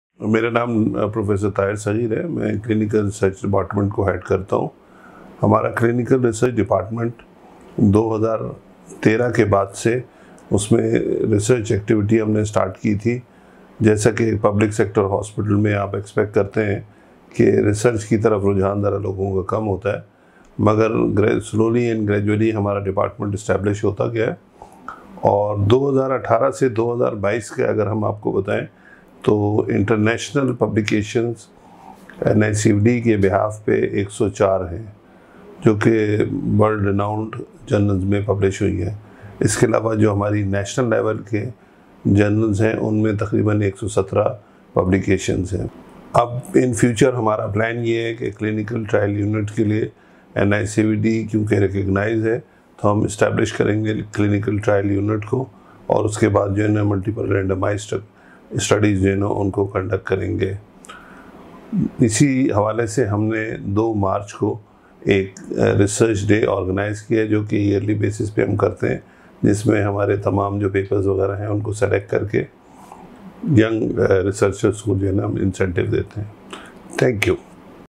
NICVD's Annual Research Day 2023: Advancing Cardiovascular Care
Our clinical research department has been making significant strides in research since 2013. At our Annual Research Day on March 3rd, 2023, we will be sharing insight into recent accomplishments and future plans for a Clinical Trial Unit and multiple randomized studies.